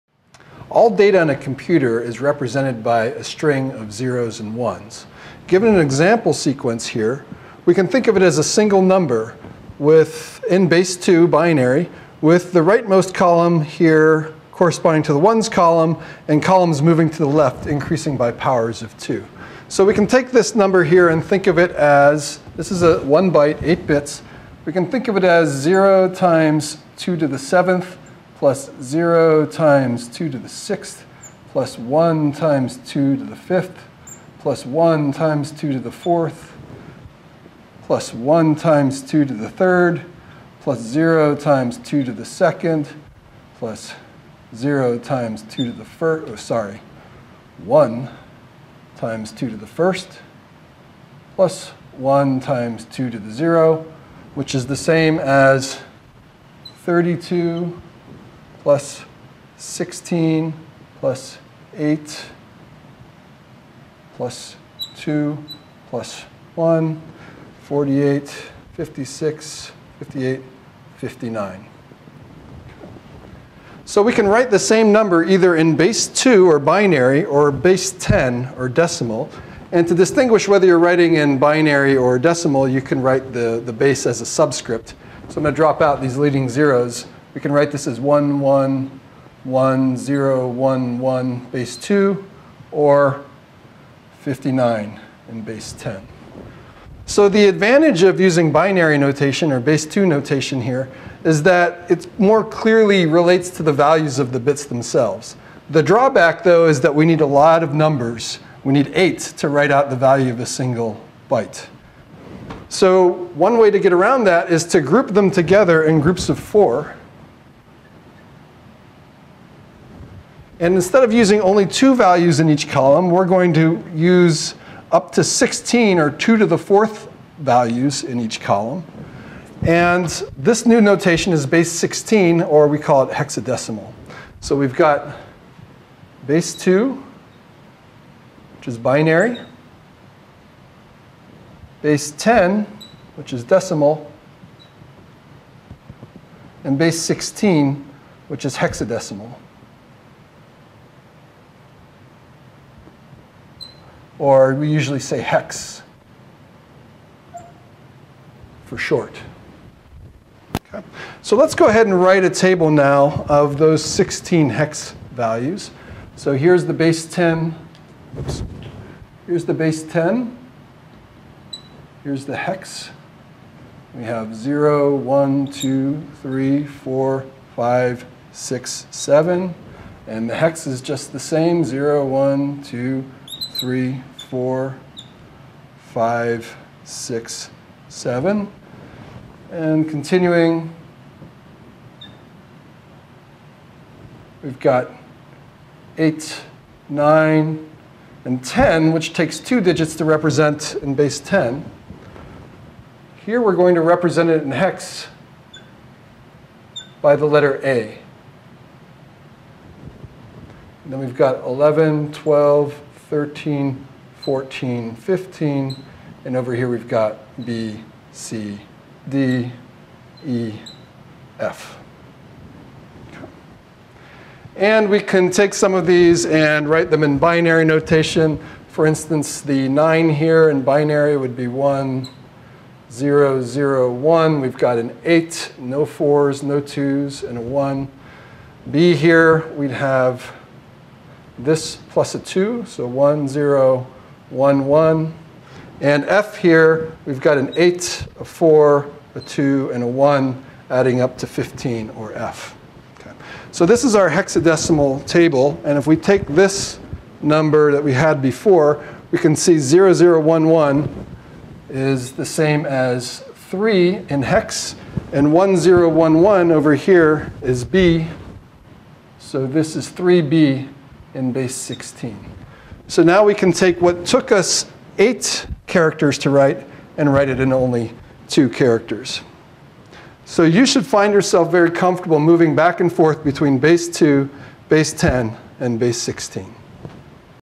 Base-2 (binary) and base-16 (hexadecimal) (Kevin Lynch)
L-comp:  Write the base-10 (decimal) number 157 in base-2 (binary) and base-16 (hex).